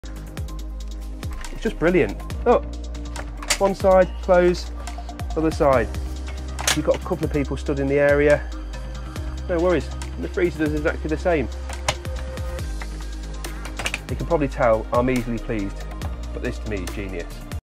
Motorhome fridge that swings both ways! 😉 ❄
The motorhome fridge that swings both ways! Ideal for a beer from either side!

Ways to make RV life easy, with RV hacks!